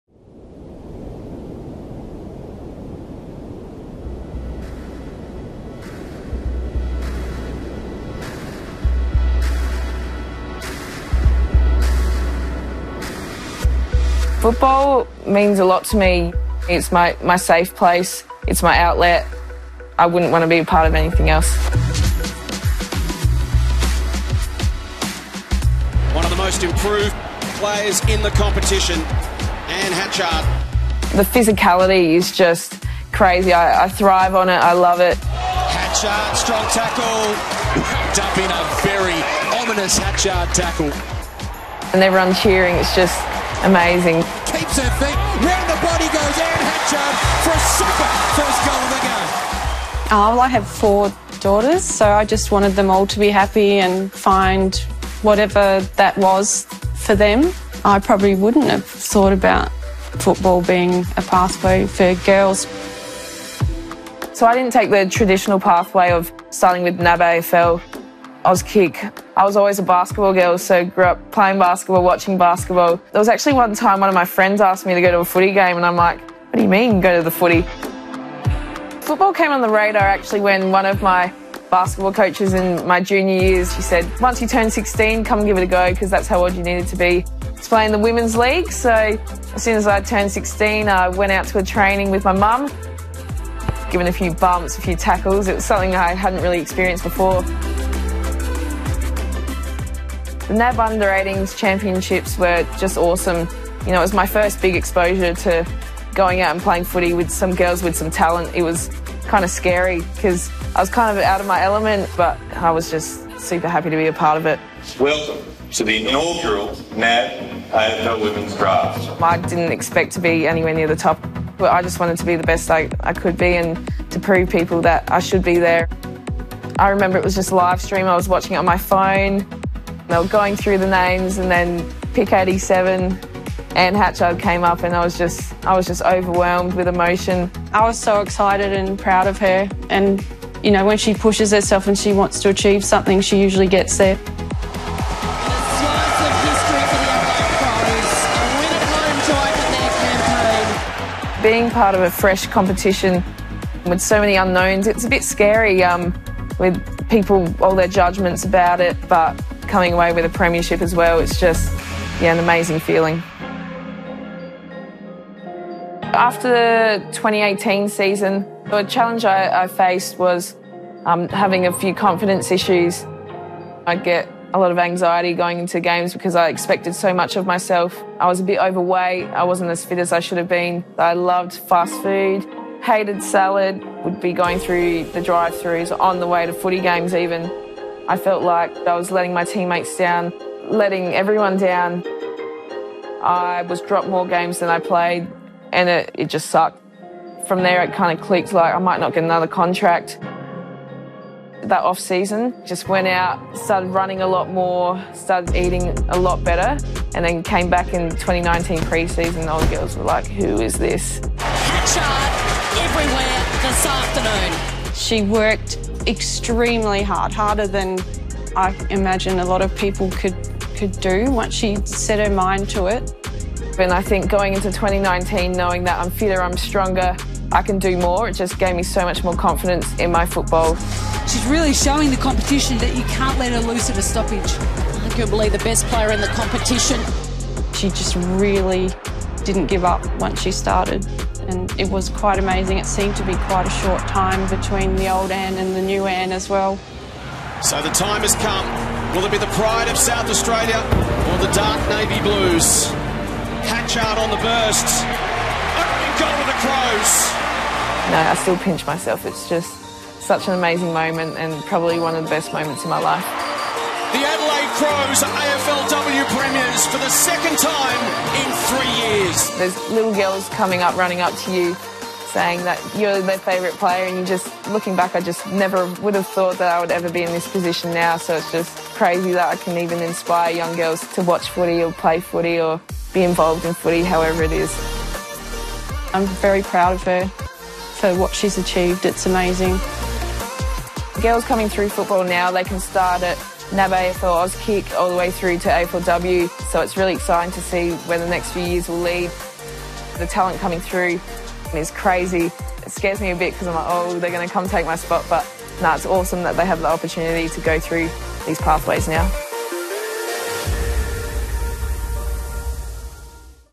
NAB Pathways: Anne Hatchard
"I wouldn't want to be a part of anything else."

We're very glad you feel that way, Hatchy! Ever wondered how one of the games' best got to where she is? Here's her story, all thanks to @NAB.